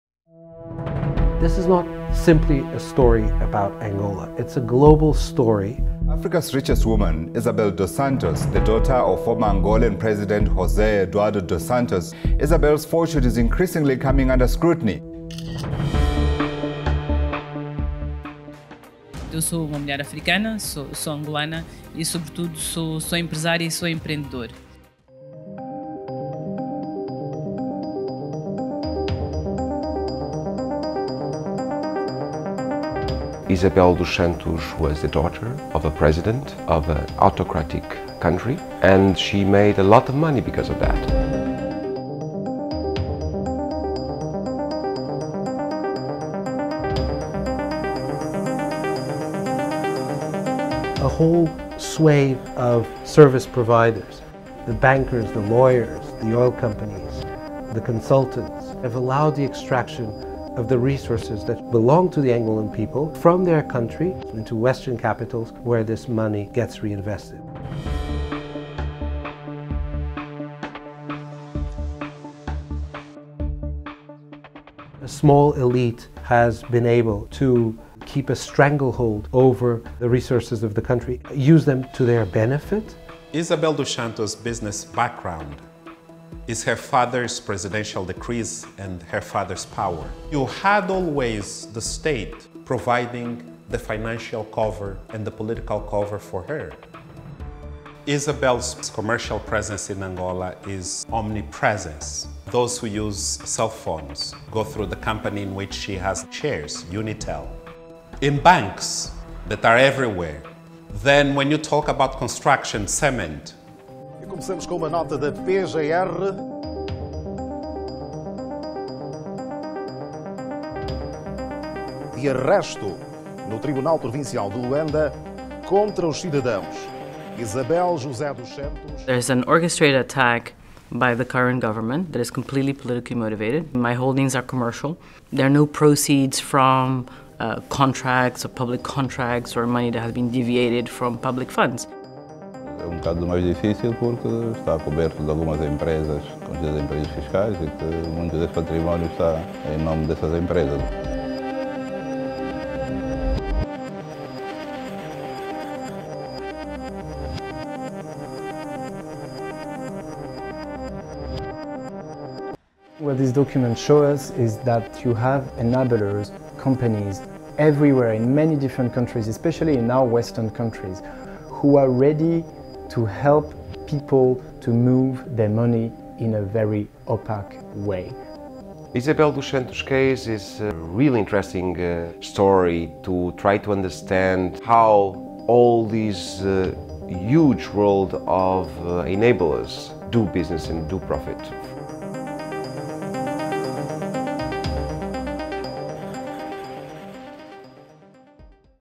Luanda Leaks: 'A global story'
The International Consortium of Investigative Journalists' latest investigation Luanda Leaks examines the inside story on how Africa's richest woman, Isabel dos Santos, made her fortune.

Based on a trove of 715,000 documents, our reporting revealed how Western financial firms, lawyers, accountants and government officials — from Lisbon to London and Luanda, from Malta to Dubai — were only too happy to help.